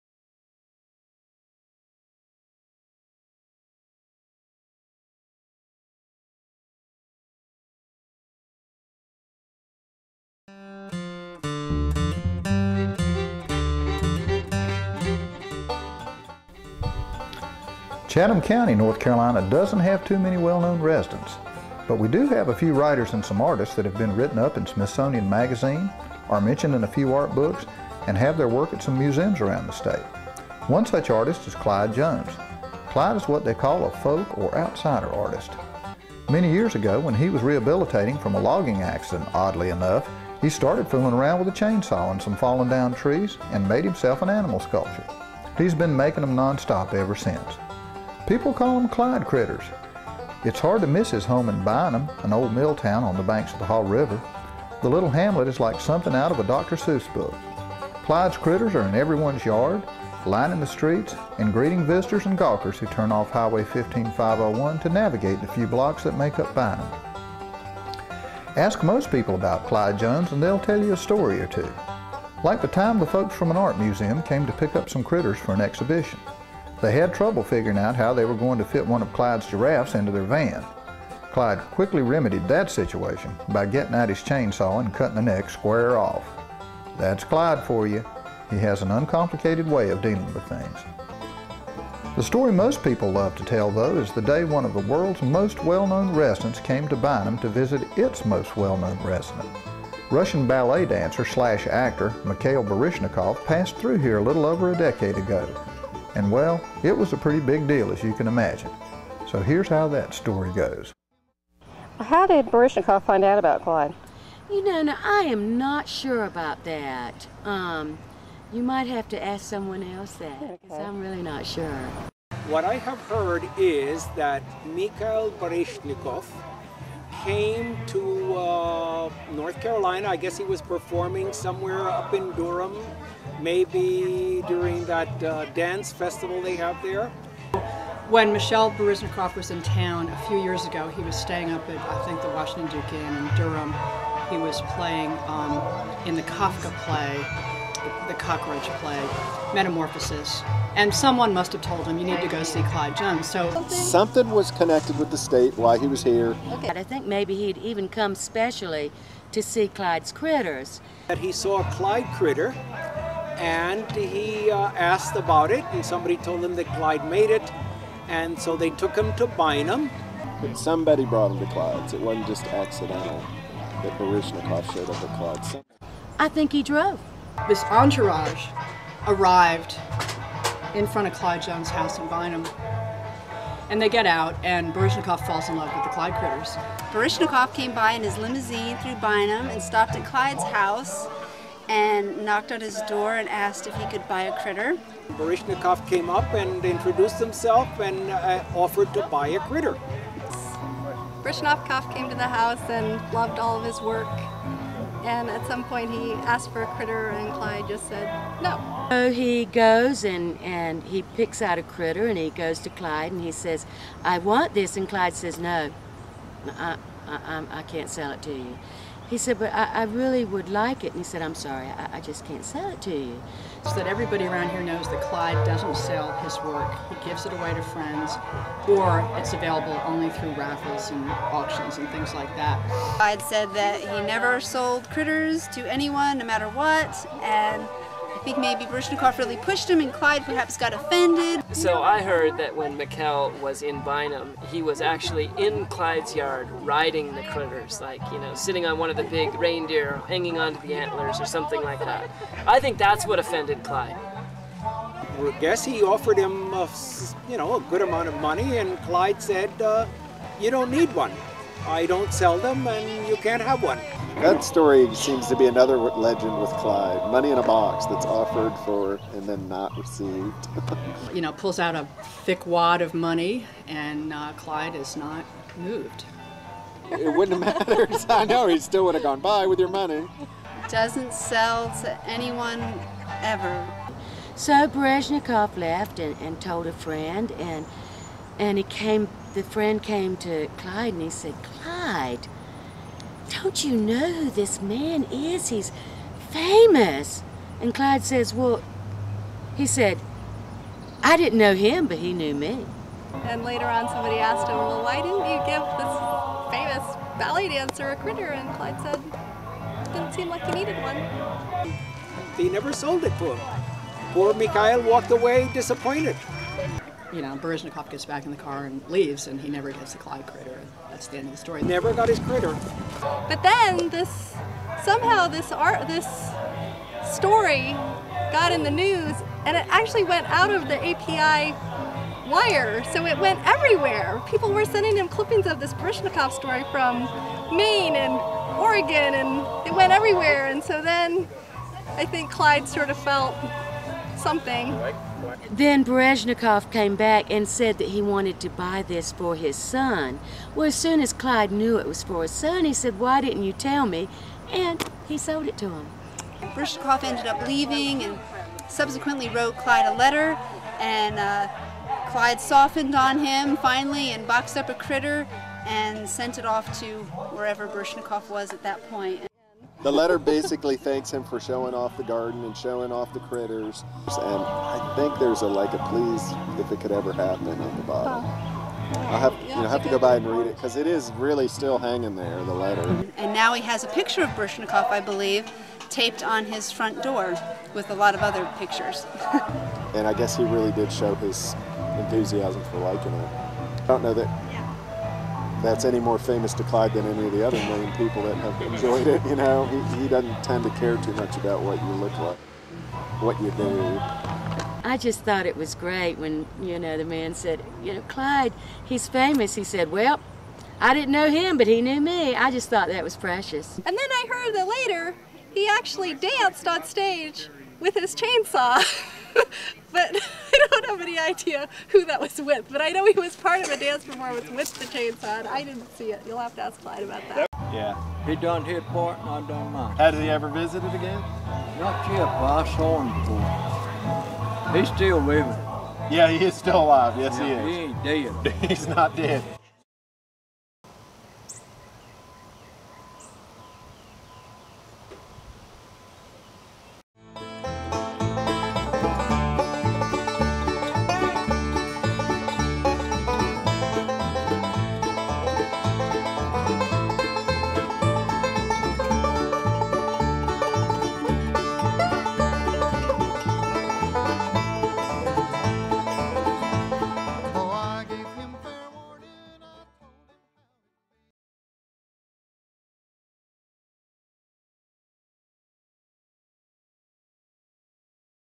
Clyde and Mikhail
Clyde Jones is a folk or "outsider" artist living in Bynum, NC.  In 2004, as a student filmmaker, Linda Booker was entertained by the different versions she heard about ballet dancer and actor Mikhail Baryshnikov's visit to Bynum and his attempt to buy a "Clyde Critter."  She filmed locals telling the story at ClydeFEST, an annual festival in Chatham County organized by ChathamArts, and edited their versions together.  Nobody knows what really happened except Clyde, but she purposely didn't include his version to illustrate how other people tell stories as local folklore and add their own embellishments.